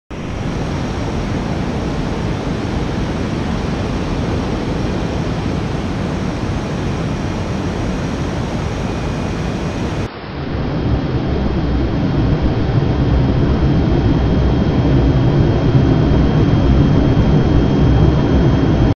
Element3D Jetstrike test video
Short clips I did testing out some renders from Element 3D with the Jetstrike model Pack.  The first one didn't turn out that well but the second looks much better.  I think I need to work on my lighting and color correction a bit.  What do you Guys think?